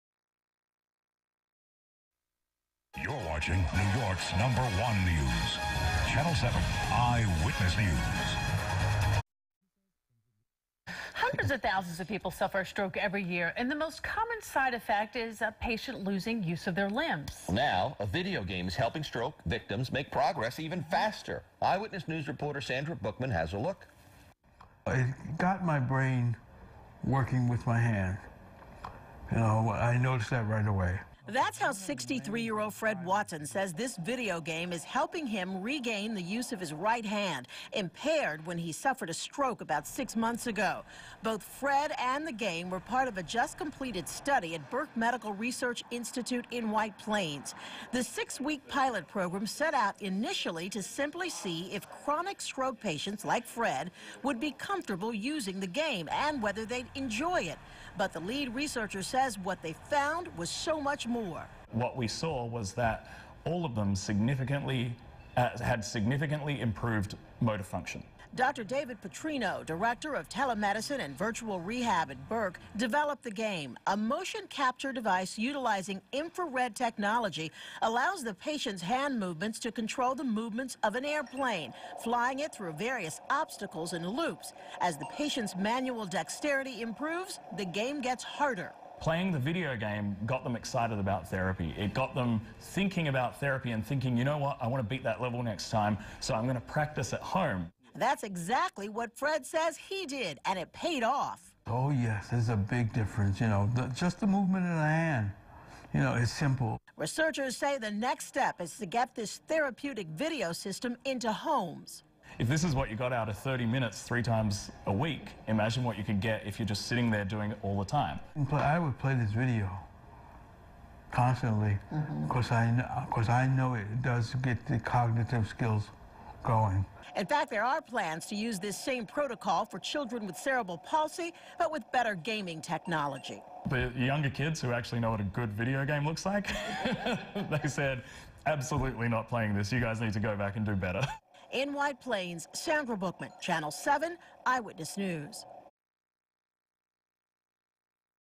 Burke Medical Research's Study on Stroke Victims & Video Games
This video is a WABC Eyewitness News story about BMRI’s Dr. David Putrino and his recently completed virtual reality/telemedicine research study for patients with chronic stroke. WABC’s Sandra Bookman is the reporter who visited campus and conducted the interviews.